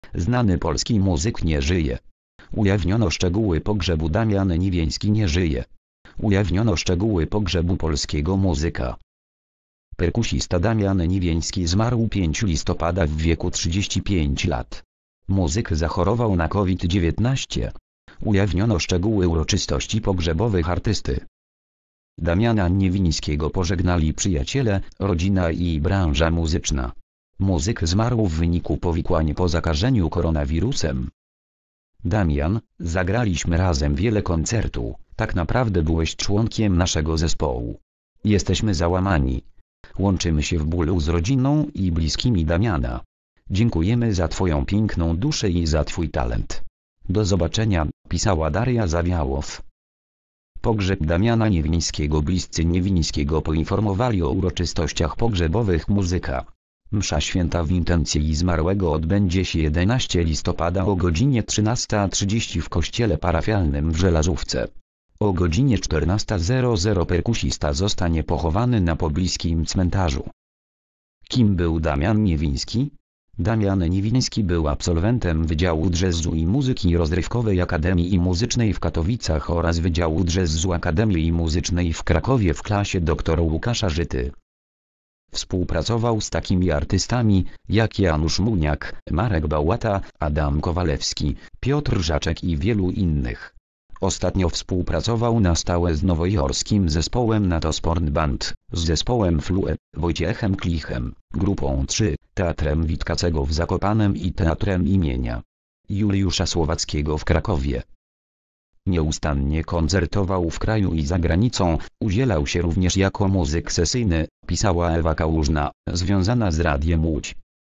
Znany polski muzyk nie żyje. Ujawniono szczegóły pogrzebu.
Damian Niewiński nie żyje. Ujawniono szczegóły pogrzebu polskiego muzyka.

  (fot. YouTube.com/axunarts - wideo zrzutu ekranu zdjęcie)